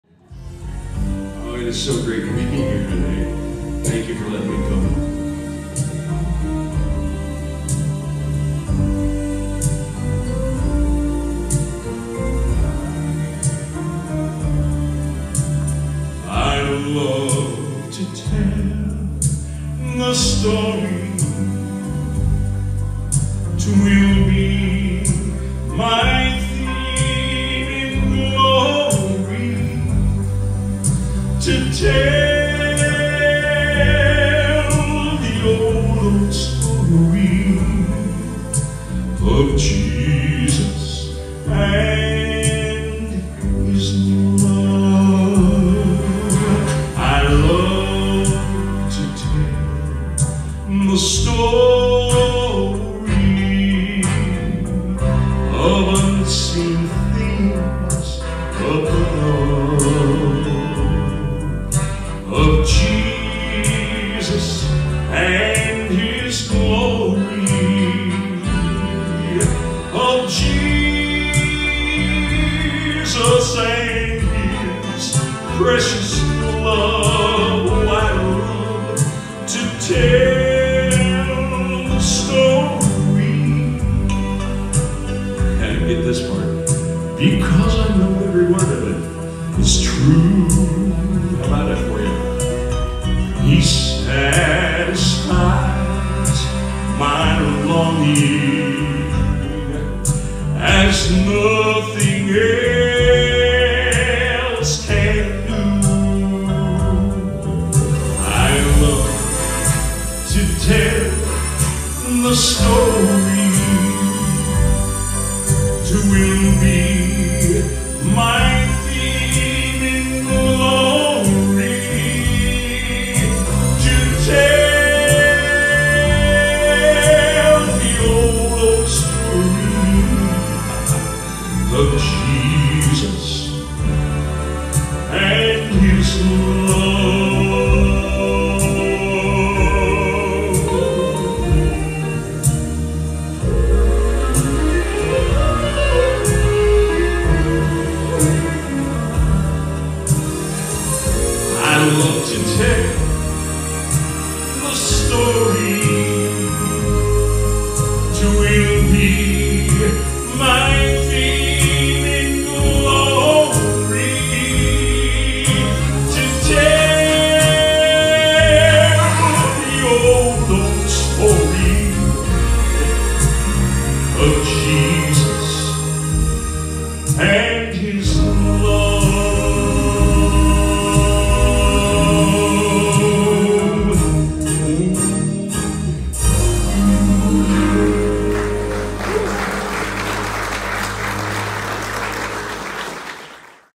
The first song I sang after almost dying
After being off for most of a year and then nearly dying from Covid, this was my first tome back on stage. It felt great and scary at the same time, I just wasn’t sure what would happen